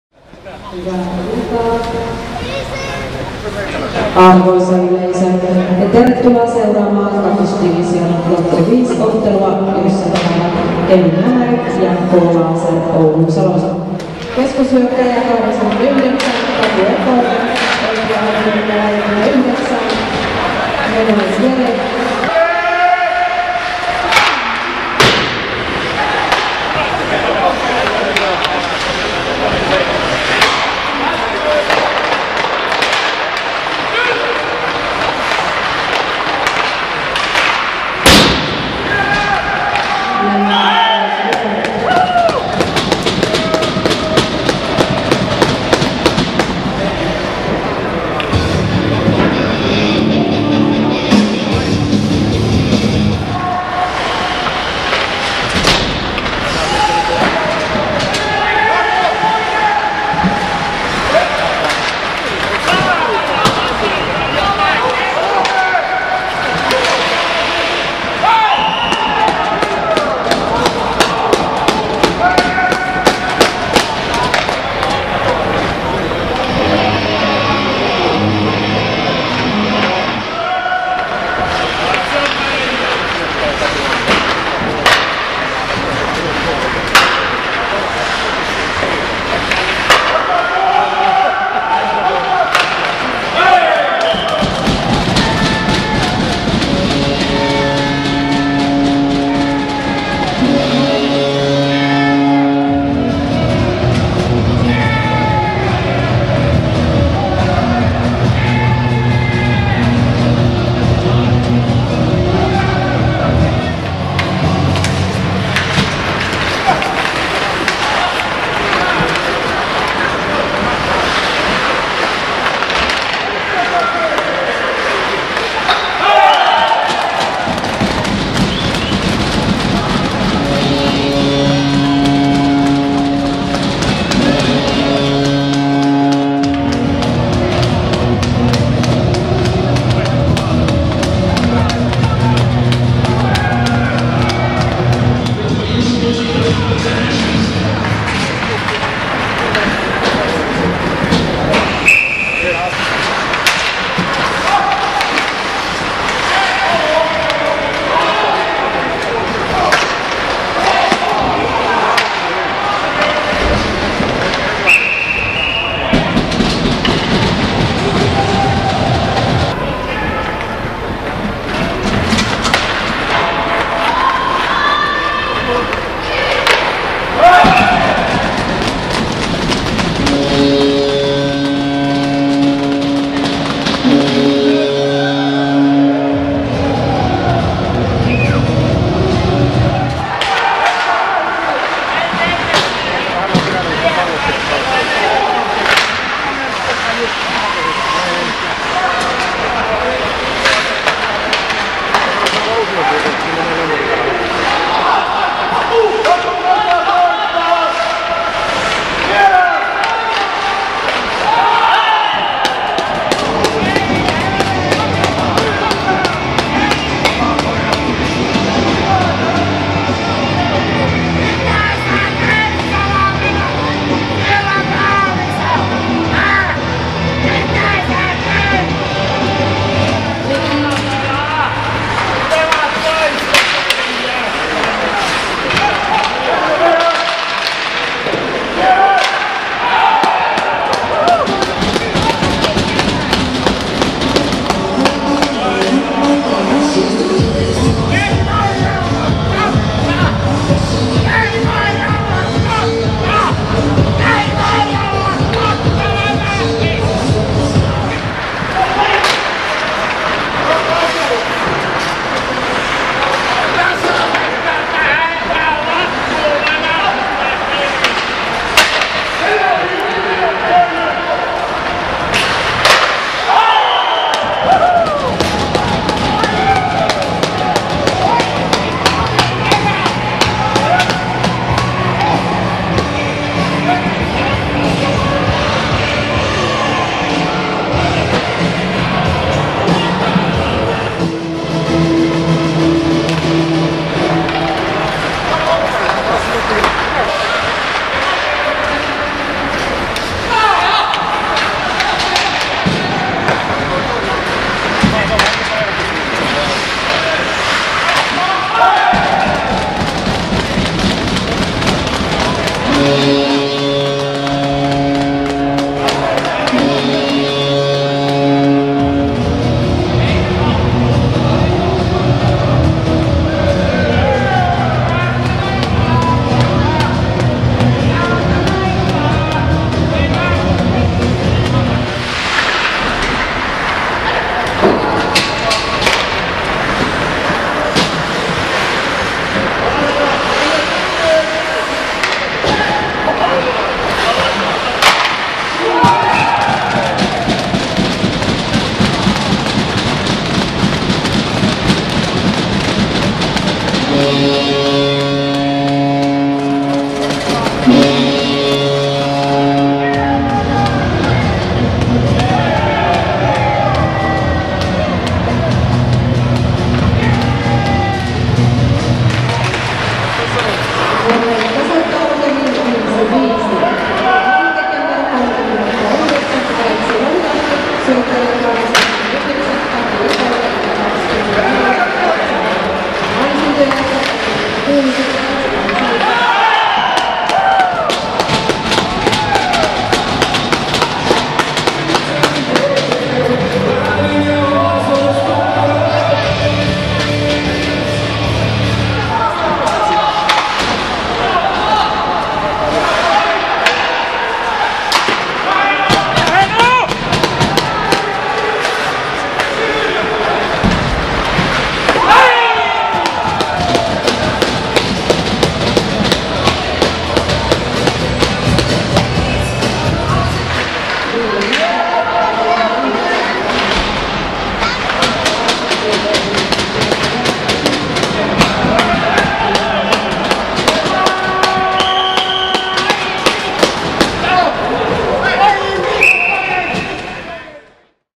Lämärit - K-Laser, 24.1.2015
Tilannekooste jääkiekon II-divisioonan ottelusta Lämärit - Kiekko-Laser. Ottelu pelattiin Kemissä Timitec Areenalla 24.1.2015.